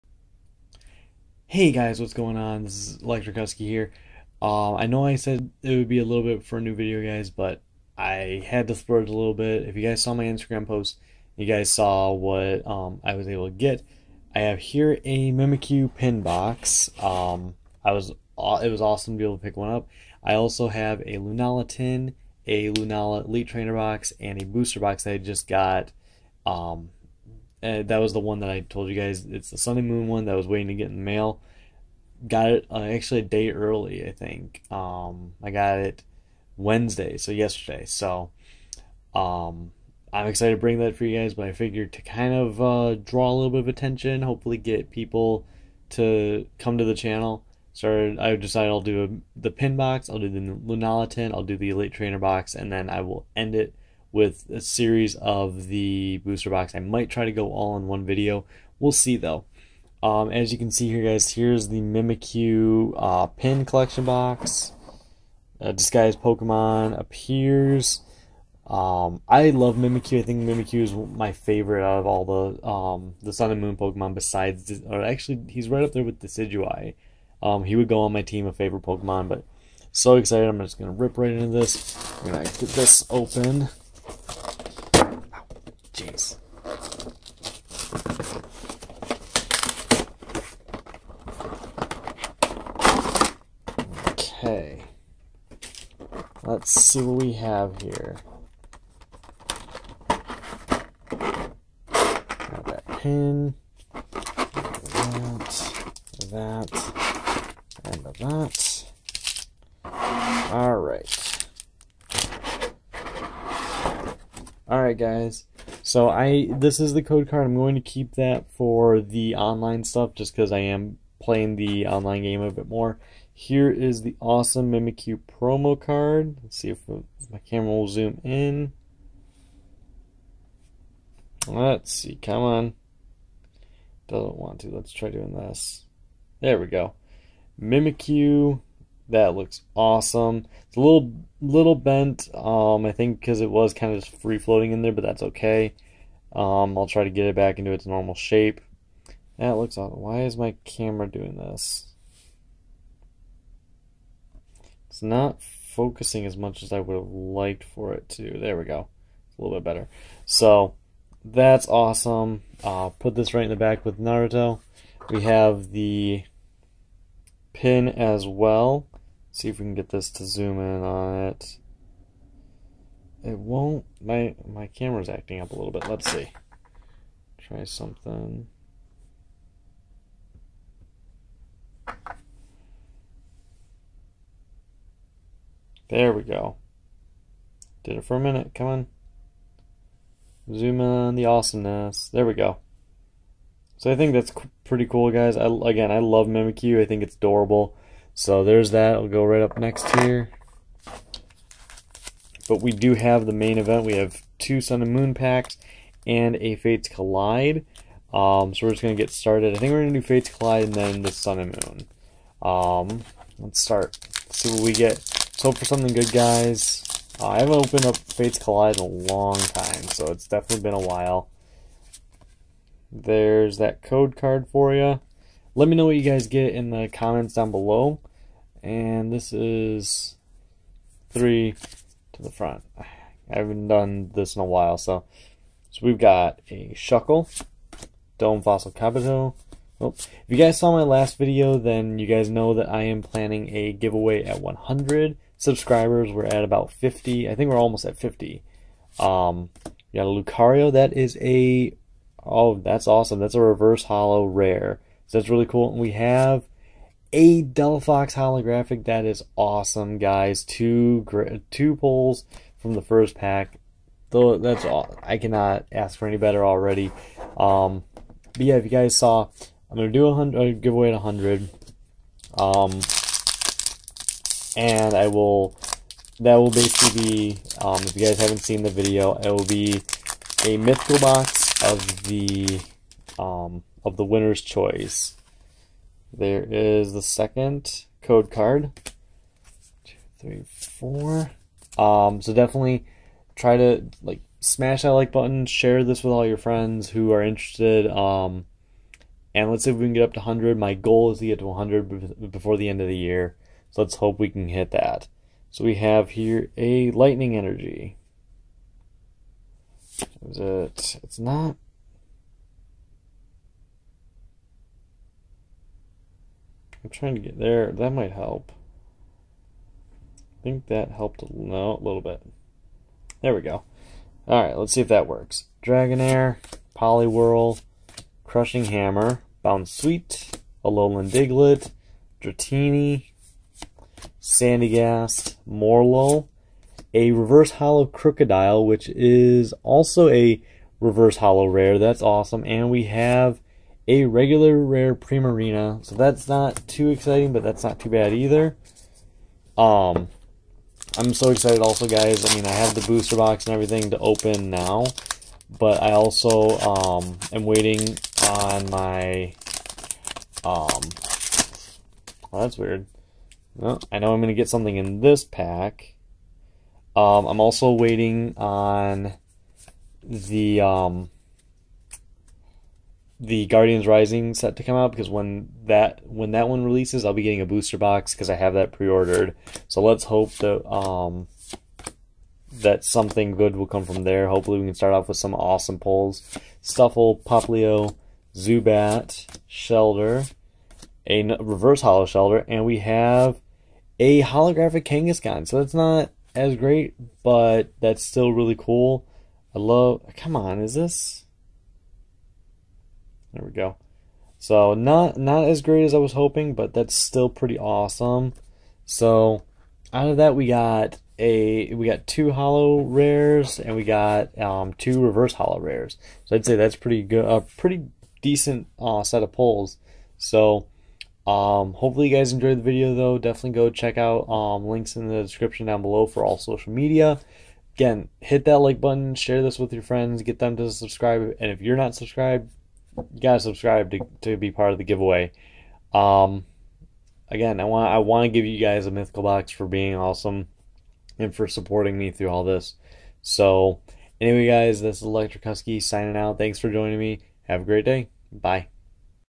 Mimikyu Box Opening!
Hey everyone! Bringing you the first in a series of videos that will end with a booster box being cracked open!

Become an official fan!
Leave a LIKE on the video if you enjoyed, and make sure to share it with friends! It helps immensely!